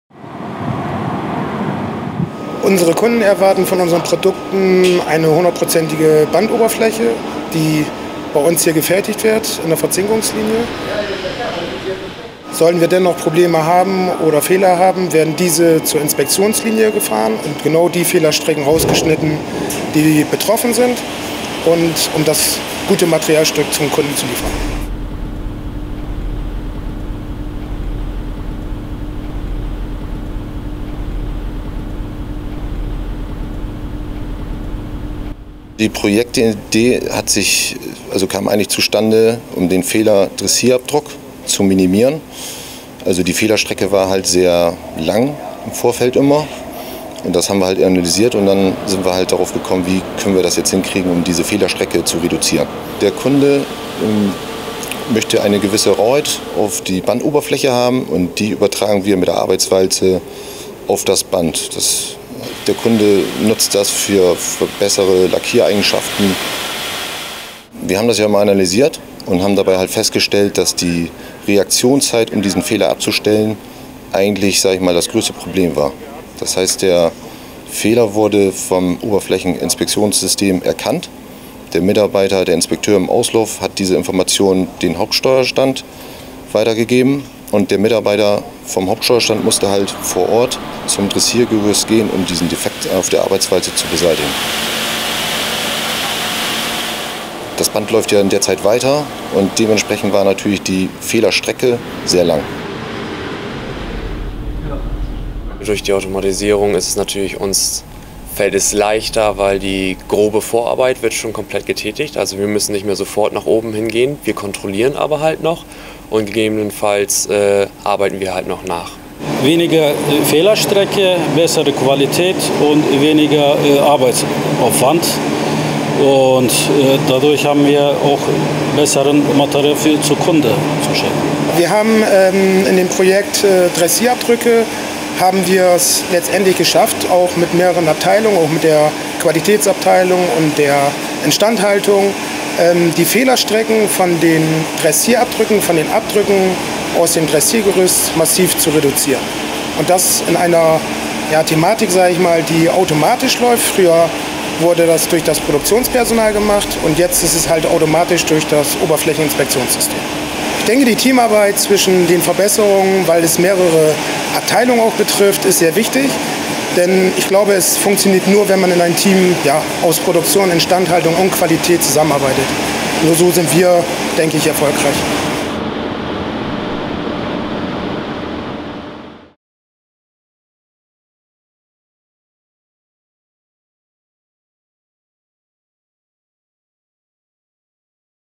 Beseitigung von Qualitätsfehlern
Die Kunden von ArcelorMittal Bremen erwarten eine hundertprozentige Bandoberflächen-Qualität der bestellten Produkte. Sollte es dennoch zu Fehlern auf dem Material kommen, werden diese in der Feuerverzinkungsanlage Bregal 1 im Bereich der Inspektionslinie schnell erkannt und beseitigt.